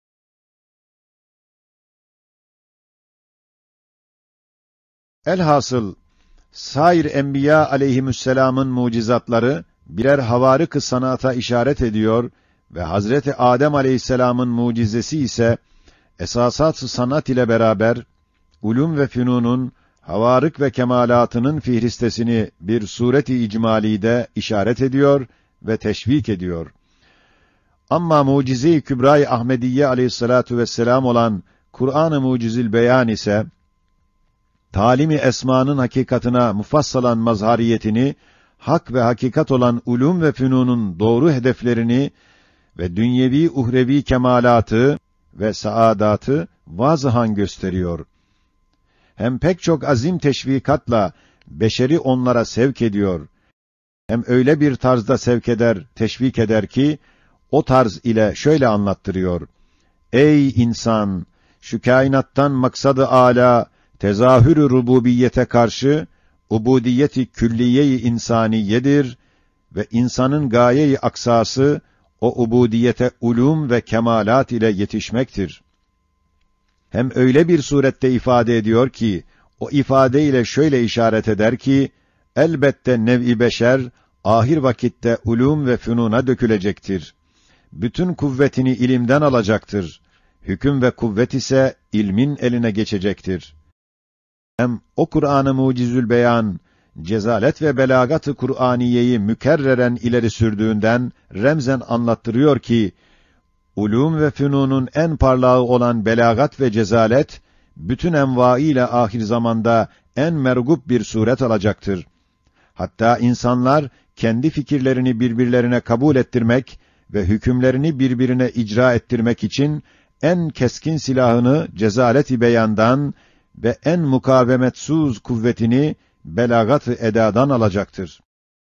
(0150) Huzmeler / Sair enbiya aleyhimüsselâmın mu'cizatları, birer havârık-ı san'ata işaret ediyor.
Külliyat : Risale-i Nur Külliyatı
Eser: Risale-i Nur'dan Hüzmeler
Bölüm: Sair enbiya aleyhimüsselâmın mu'cizatları, birer havârık-ı san'ata işaret ediyor.
Müellifi : Bediüzzaman Said Nursi